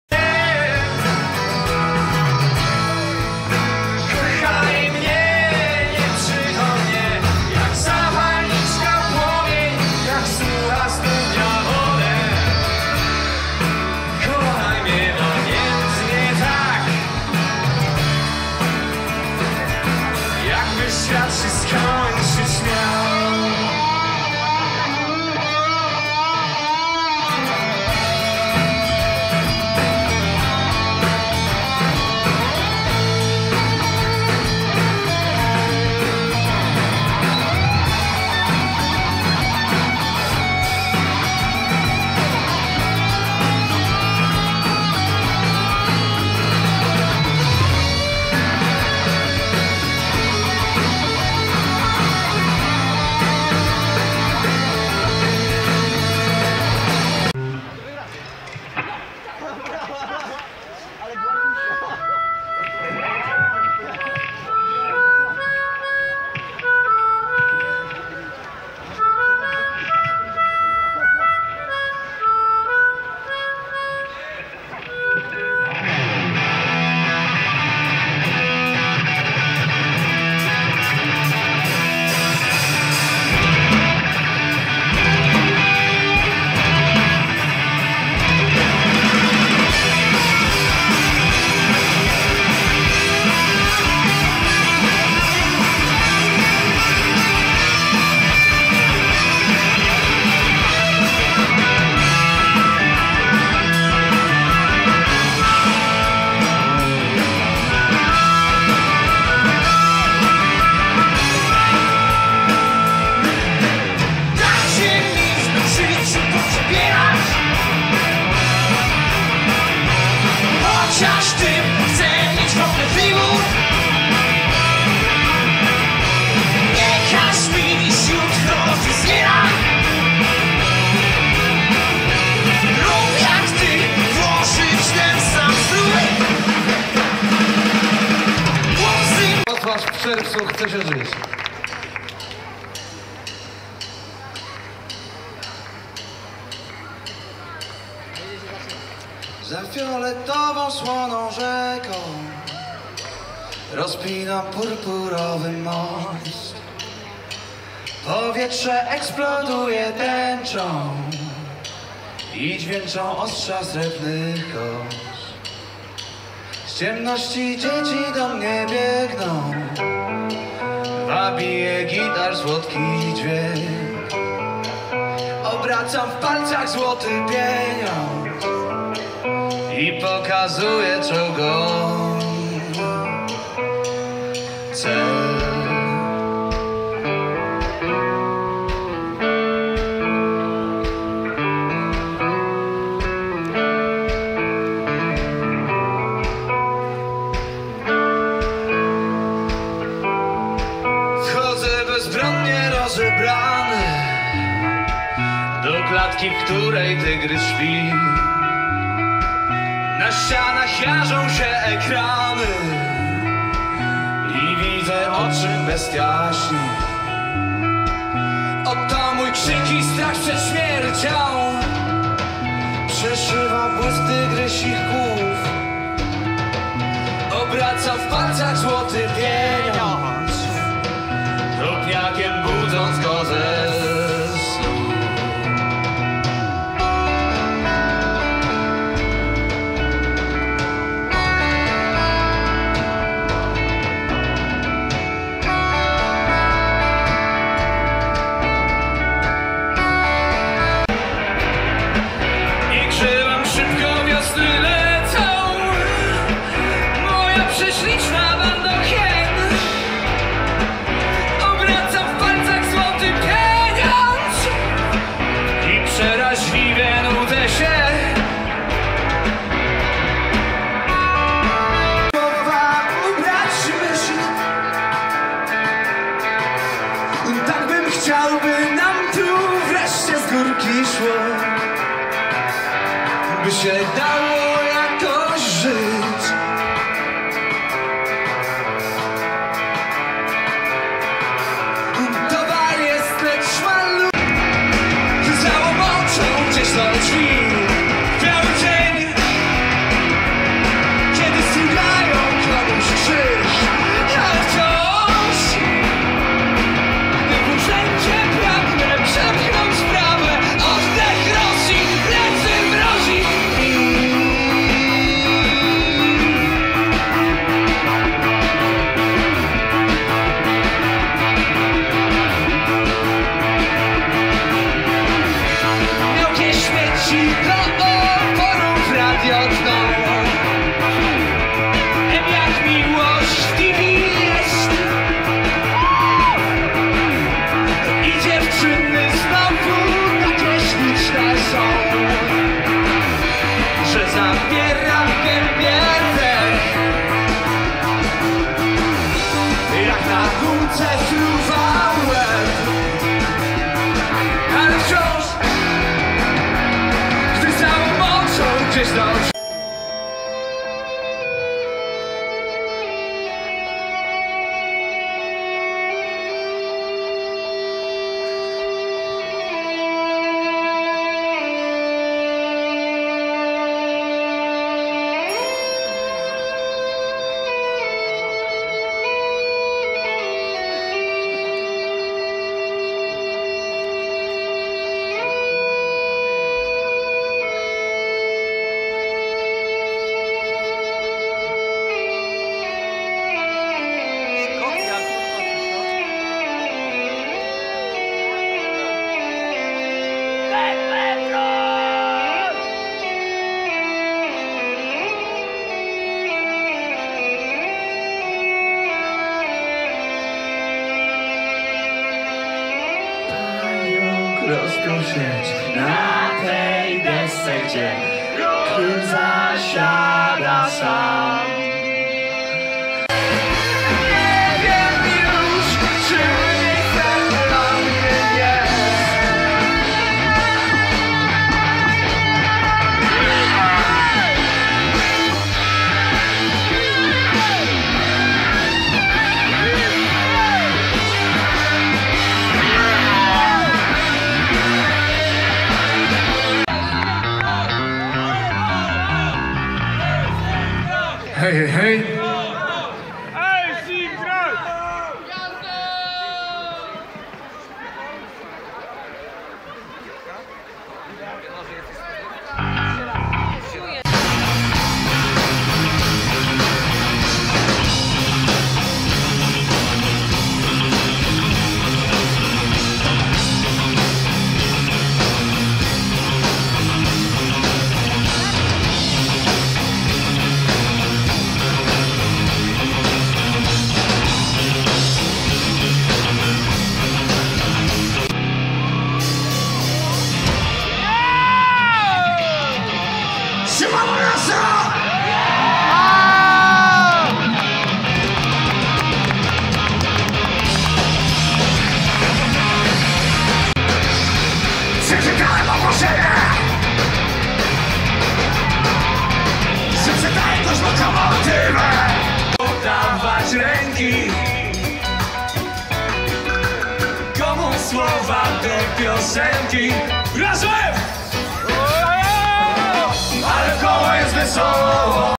Nowe brzmienie Perfectu - Wrocław 10.06.2011
Lady Perfect, Trzynasta w samo południe, Muchy, Acid Drinkers i Maleo Reggae Rockers

Grali jeszcze (niezarejestrowani tutaj): Maciej Maleńczuk & Psychodancing oraz Perfect :)

Skupiłem się głównie na zespołach mniej znanych, gdyż uznałem, że takich np. Acidów to masa innych ludzi pewnie będzie filmować. Nie myliłem się - na YouTube można znaleźć ich utwory w całości ;) Żałuję, że nie mogłem nagrać więcej Maleo - skończyło się miejsce :(